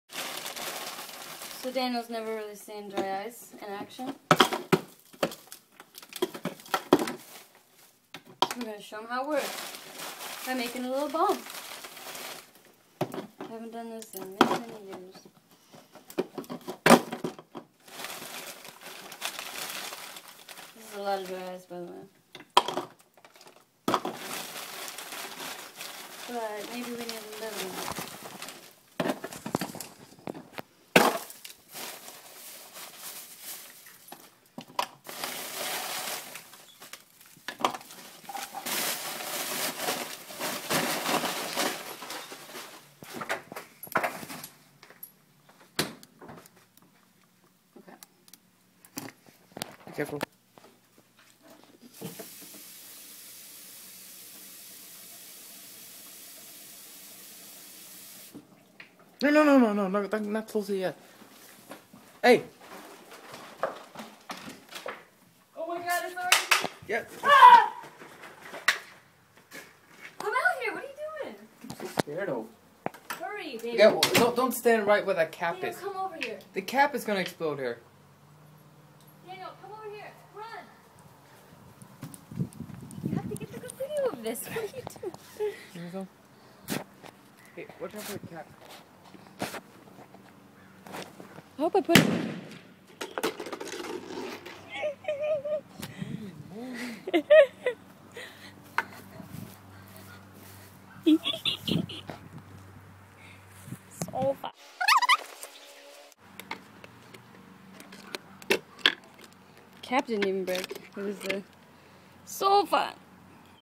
Carah + Daniel Dry Ice Bomb
Daniel has no idea what dry ice is. So we bought some, and I showed him how to make a little bomb.  If you try this please be careful.  I kind of(lol) knew what I was doing.